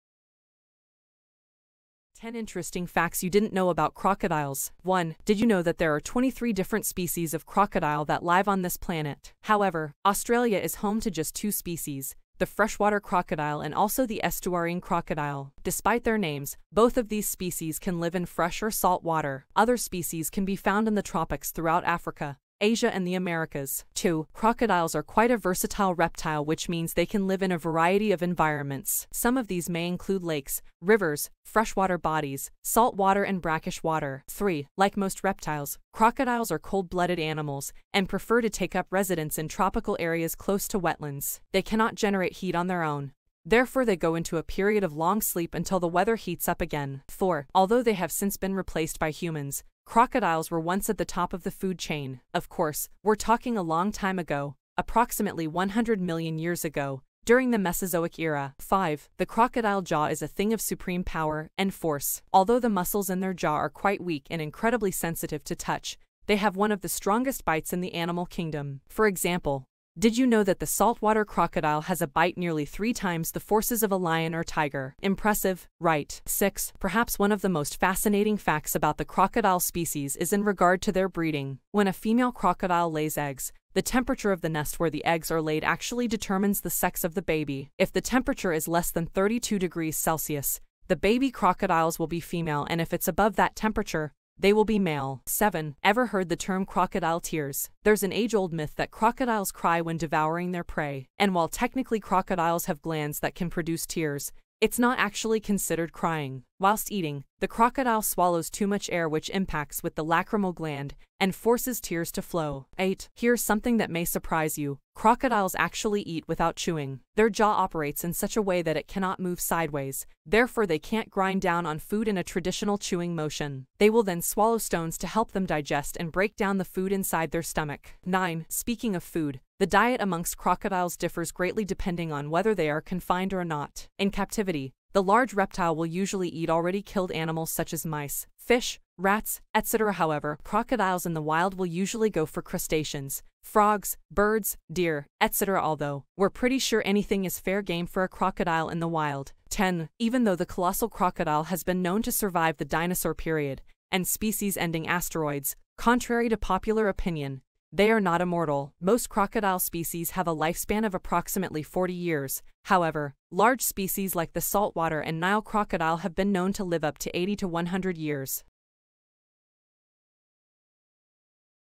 TOP 10 Amazing Crocodile Facts 🐊 | Facts About Crocodiles (2022) | Pets and Us
Crocodiles have the strongest animal bite in the world, In this video we're taking a deeper look at crocodiles to reveal some amazing things you probably didn't know about them. 10 Interesting and Amazing facts about Crocodile.

Welcome to PETS & US, enjoy my pets videos - have fun in learning amazing facts of different animals and how to take care of them. This channel is all about animals and pet lovers.